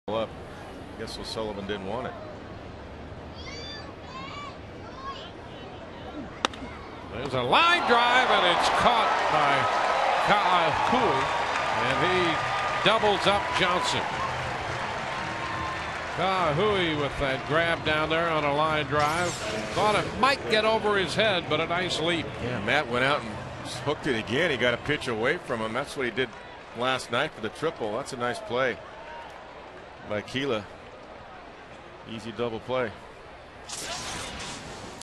2010/10/03 Ka'aihue turns two
TB@KC: Ka'aihue snags the liner, steps on first